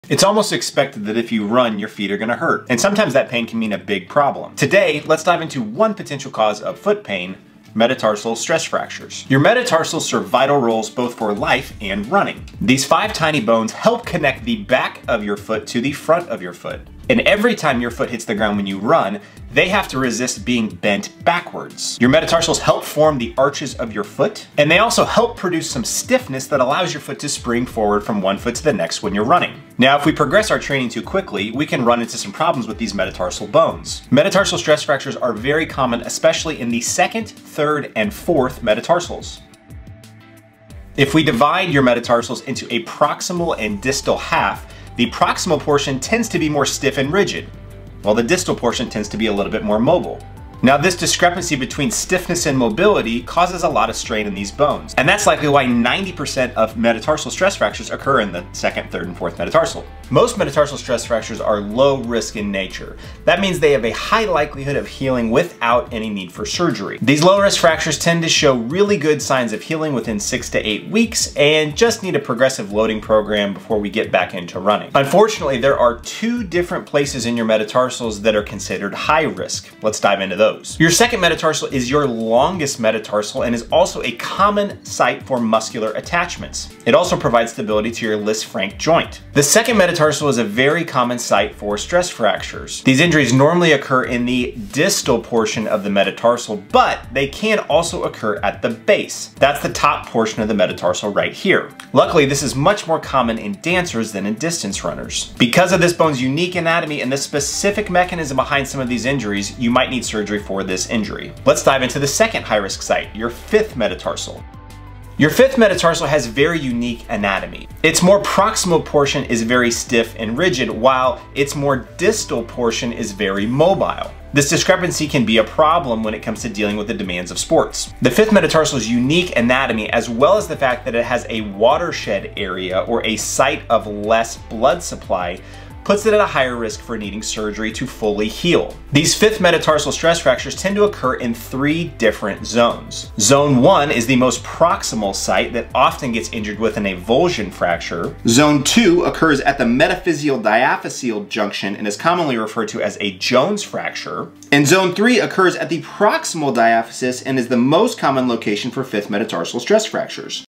Metatarsal Stress Fractures | Causes, Diagnosis, & Treatment
0:10 Metatarsal Function During Running
0:34 Metatarsal Biomechanics
0:56 Most Common Stress Fracture Locations
1:15 High Risk Metatarsal Stress Fractures
2:40 5th Metatarsal Zones